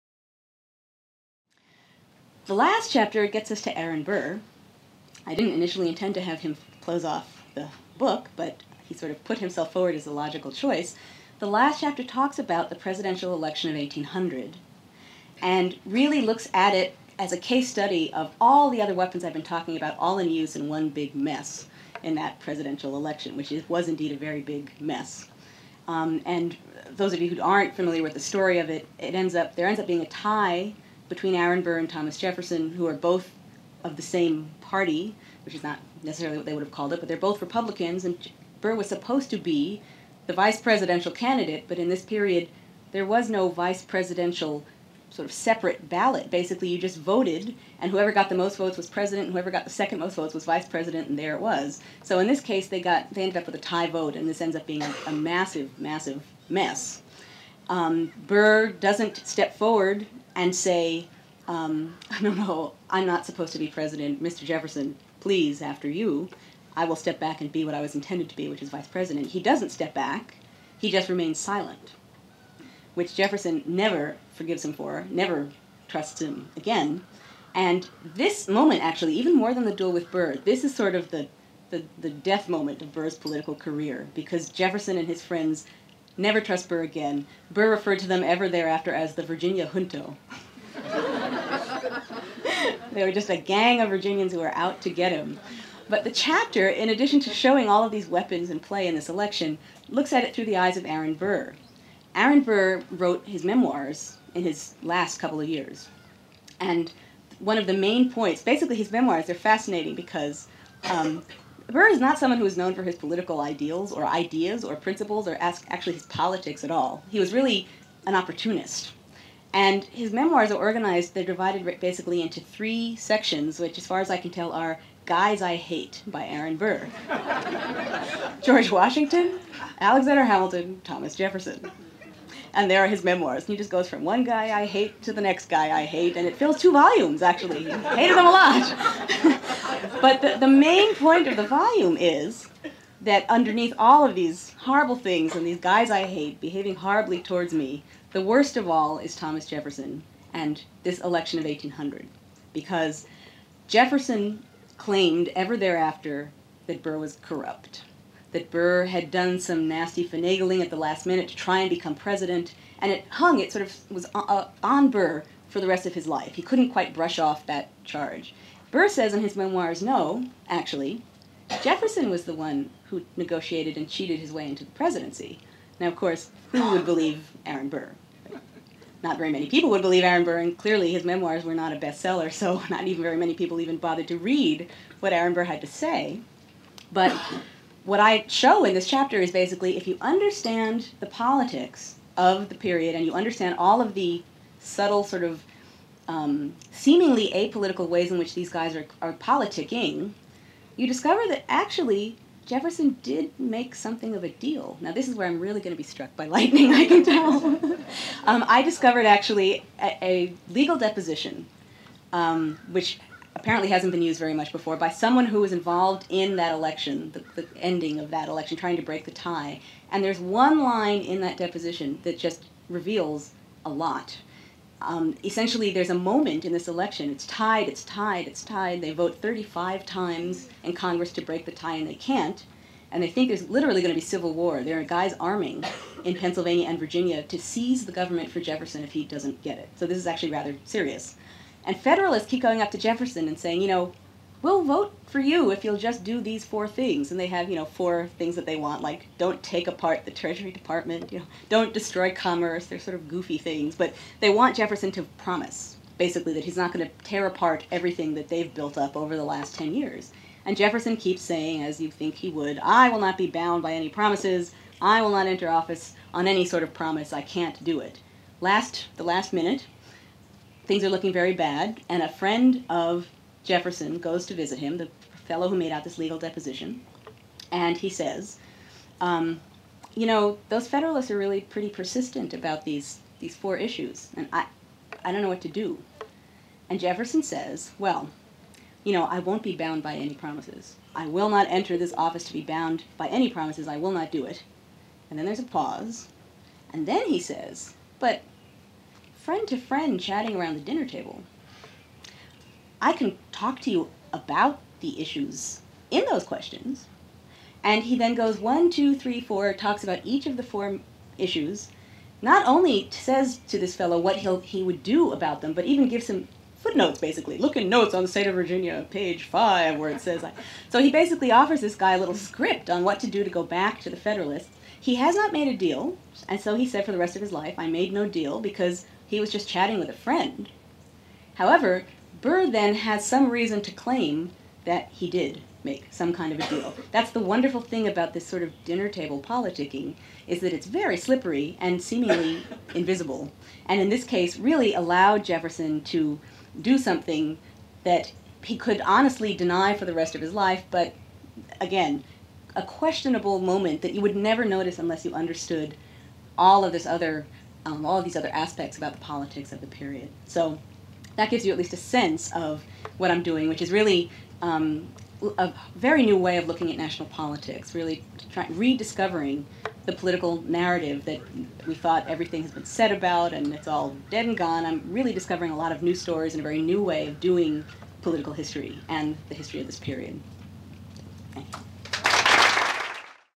Aaron Burr: "Guys I Hate"
Professor Freeman ends her talk by relating the events of the Election of 1800 and the enmity that arose between Jefferson and his "running mate."